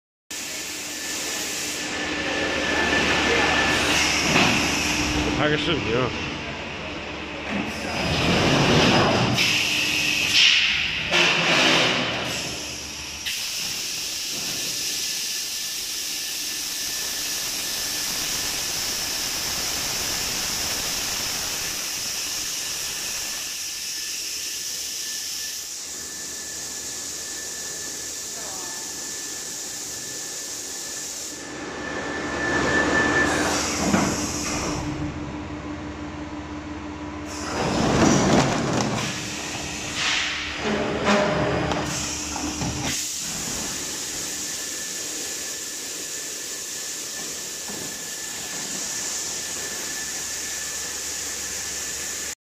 Bento tray thermoforming machine pp container
Semi automatic thermoforming machine for producing the bento tray pp container.In China,the "railway food container" is very popular,and it is in high demand.I believe that the catering products are also in great demand everywhere.Our machine could produce different sizes and shapes of the bento tray,fully automatic machine and semi automatic machine all at your choice!
Angel Zhou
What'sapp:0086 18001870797
Wechat:Angelzhou93